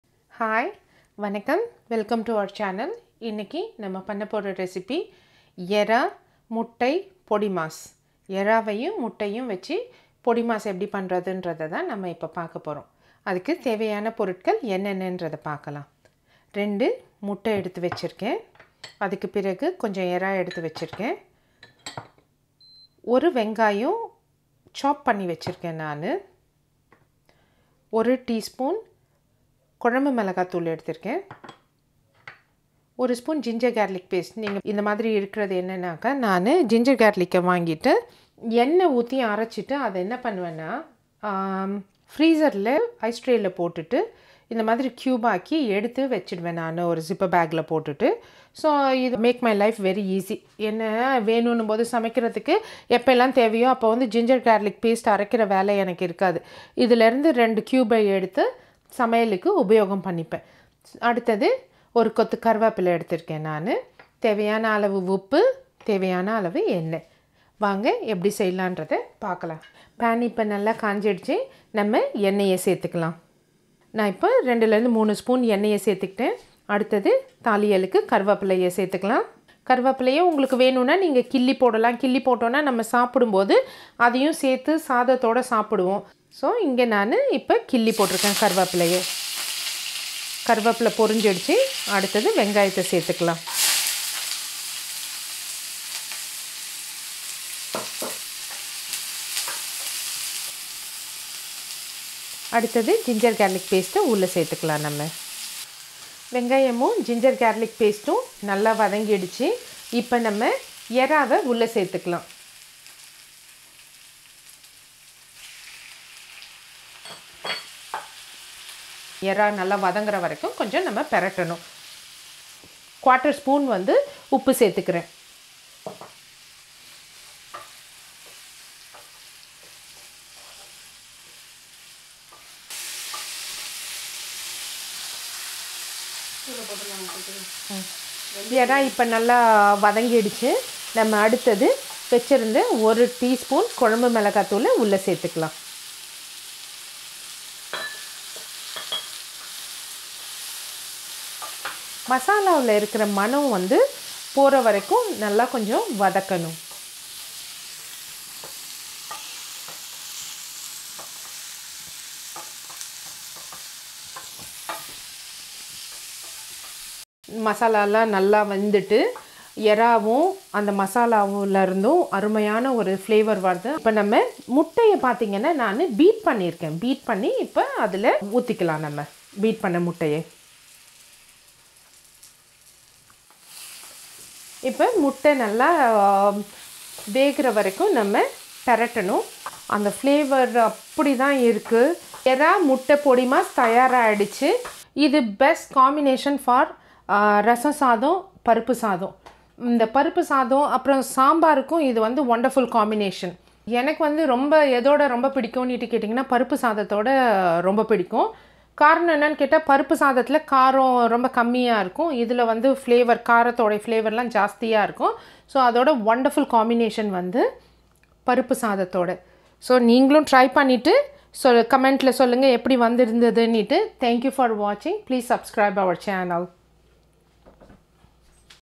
Anjappar Style Eral Muttai Podimaas| Egg Shrimp Podimas in Tamil | Eraa Muttai Poriyal
Ingredients: Egg 2, Shrimp (Prawn) 10 - 15, Onion 1, Curry Powder 1tsp, Ginger Garlic Paste 1 tsp, Some Curry Leaves, Oil 3 tbsp, Salt 1 tsp.